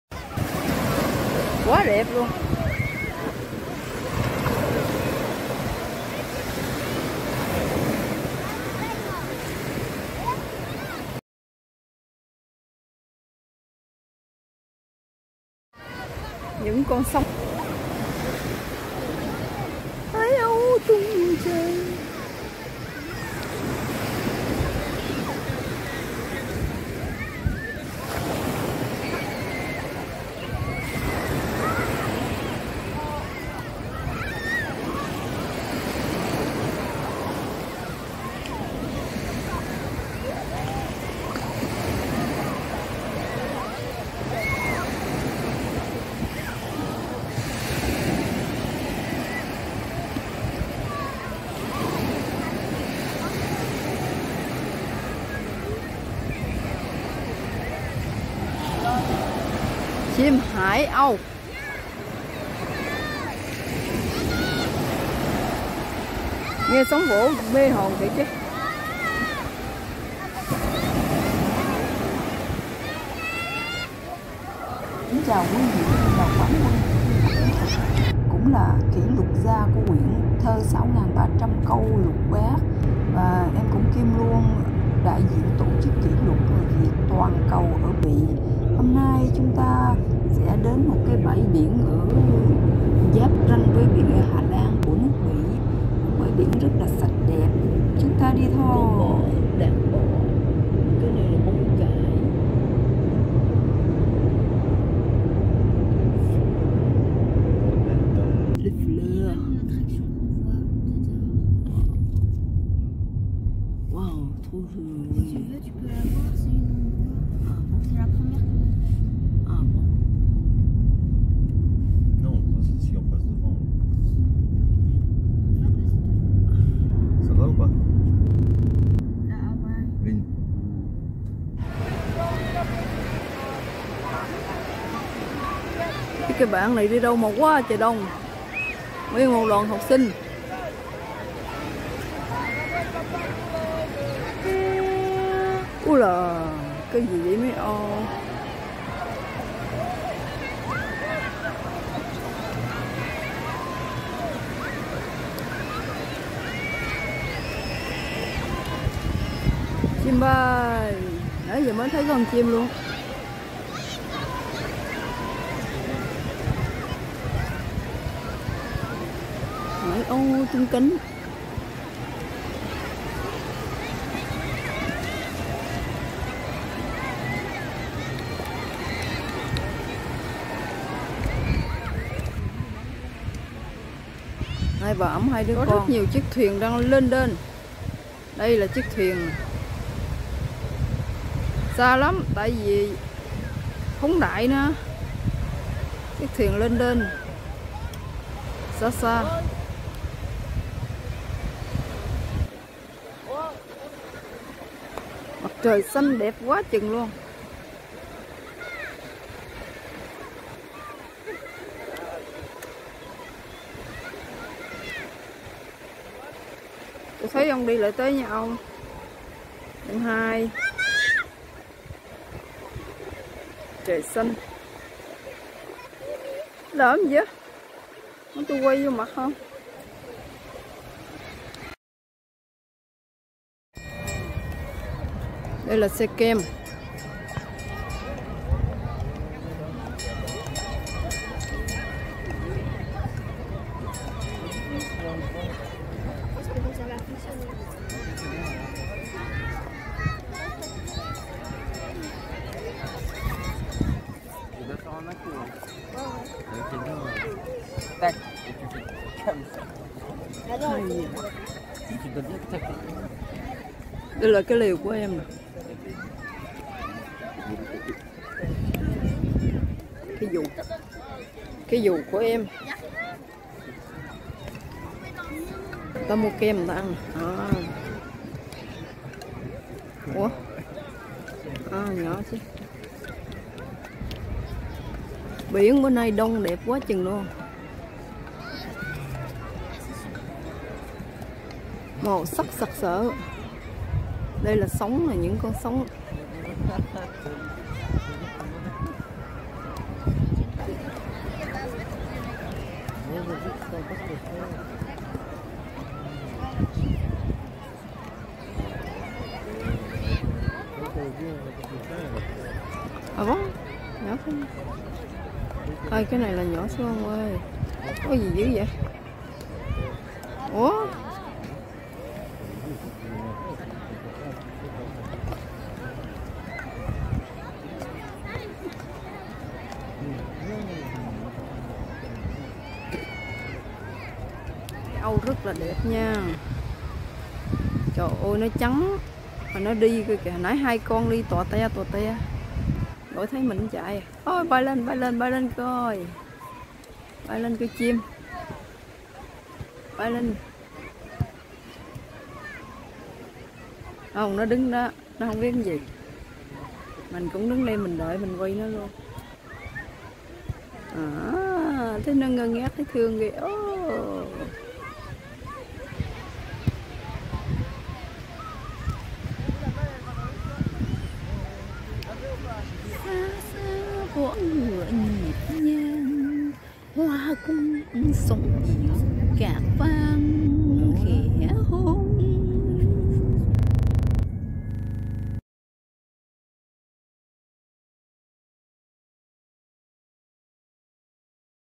Bãi Biển Bỉ Trong Xanh, ngắm mấy cô gái đẹp quá. La lumineuse plage de Coxyde 'Danh Thắng Bỉ 2
Bãi Biển Bỉ Trong Xanh, ngắm mấy cô gái đẹp quá. La lumineuse plage de Coxyde
Xa xa vó ngựa nhịp nhàng,
Hòa cùng sóng biển cát vàng khẽ hôn.
Chiều tà nắng chiếu biển hồn,
Long lanh nước biếc bồn chồn lòng ai!?